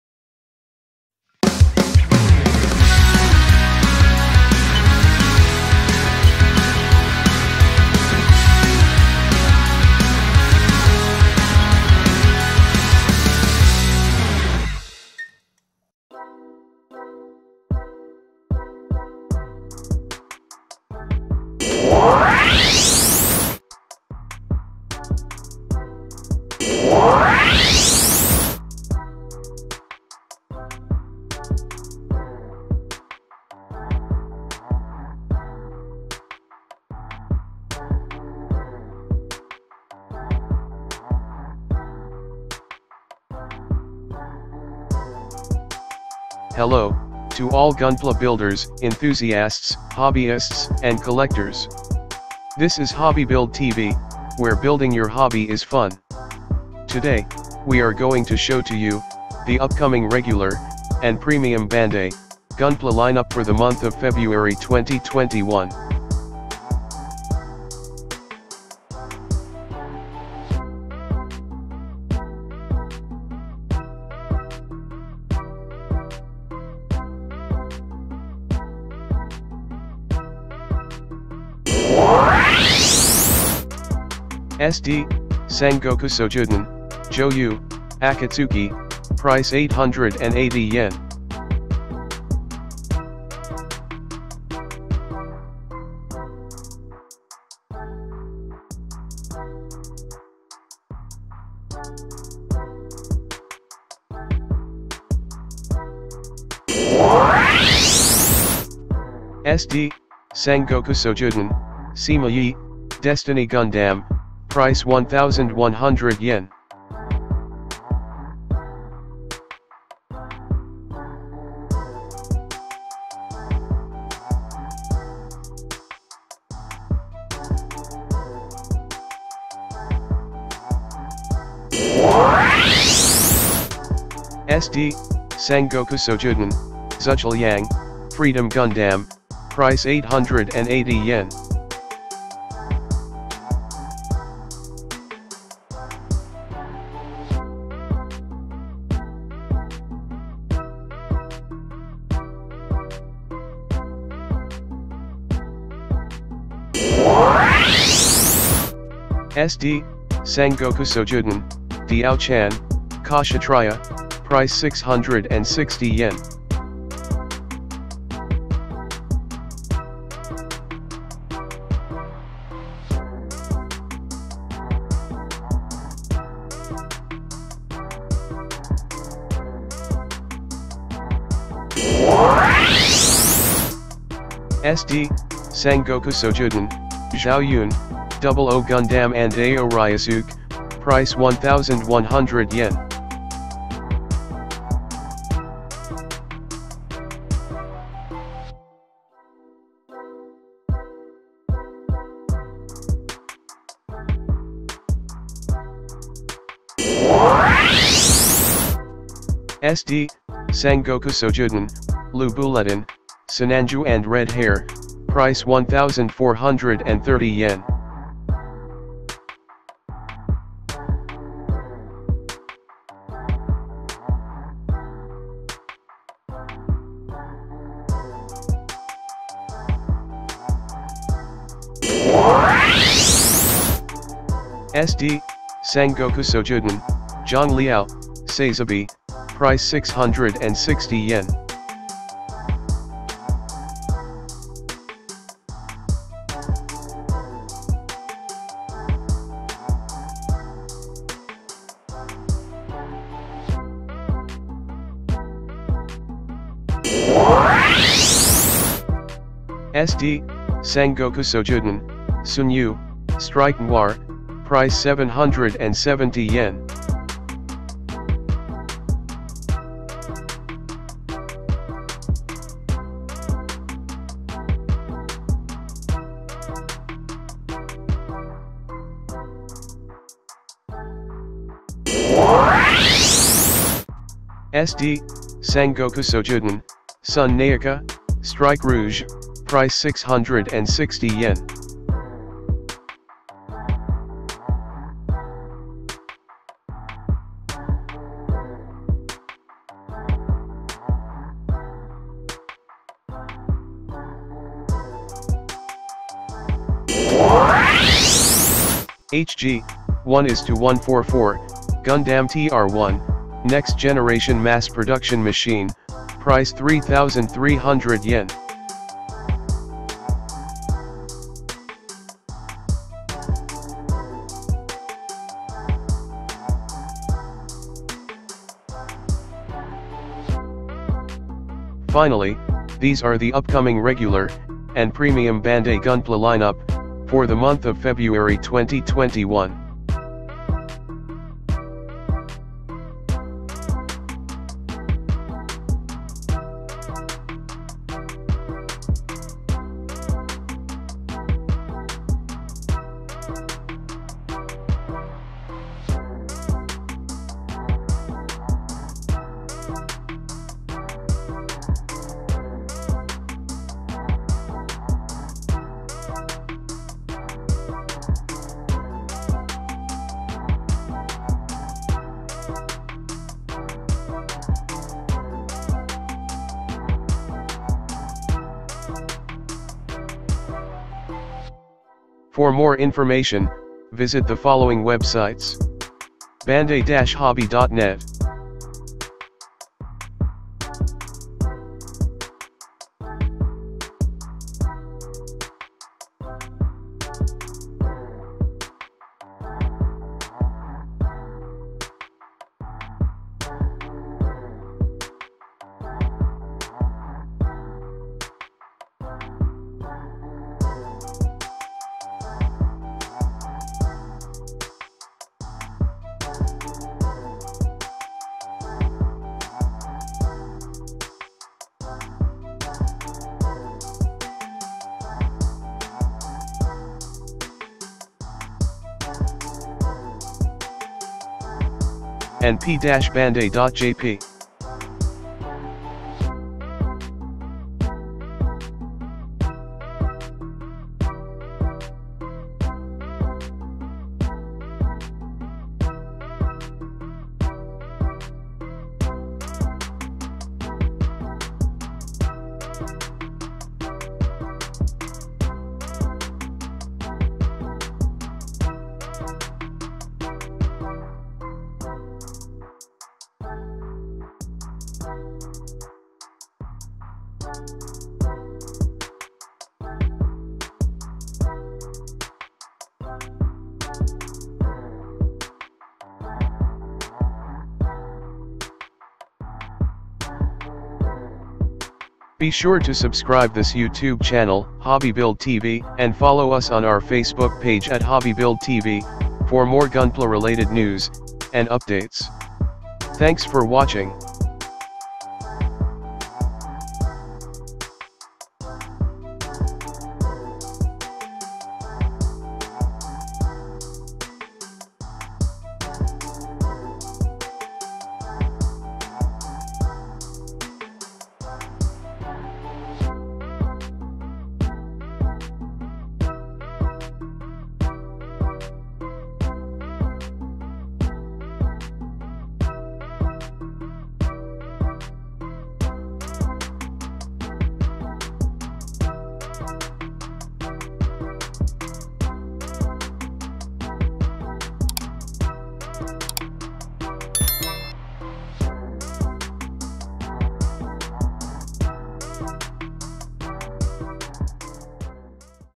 Gunpla Lineup February 2021 | HobbyBuild TV
Showing to you the latest gunpla lineup for the month of February 2021

credits ro bandai-hobby.net and p-bandai.jp for the images and informations.

for more gunpla related news and updates.